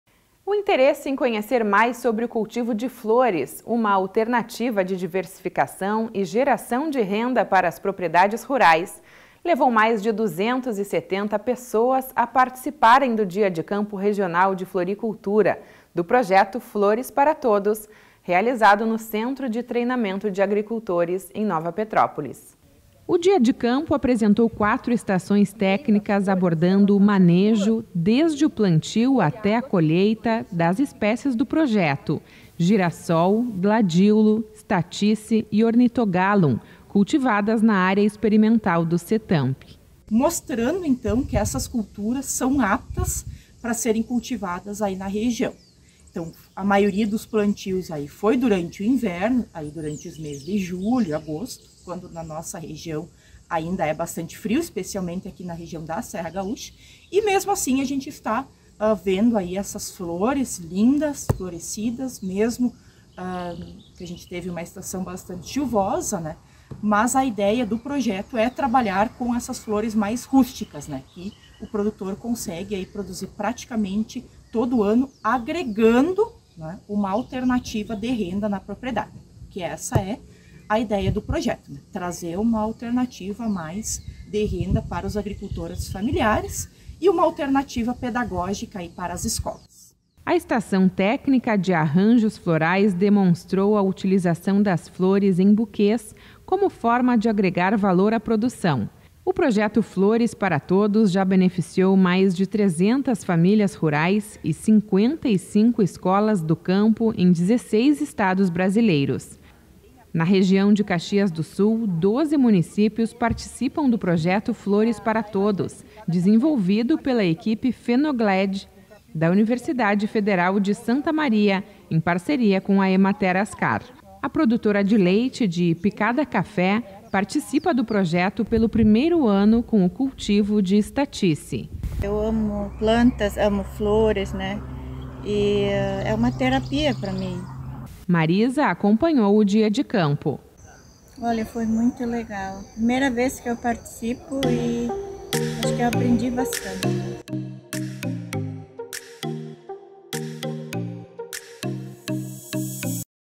Dia de campo mostra potencial da floricultura para a agricultura familiar - Rio Grande Rural
O interesse em conhecer mais sobre o cultivo de flores, uma alternativa de diversificação e geração de renda para as propriedades rurais, levou mais de 270 pessoas a participarem do Dia de Campo Regional de Floricultura, do projeto Flores para Todos, realizado no Centro de Treinamento de Agricultores (Cetanp), em Nova Petrópolis.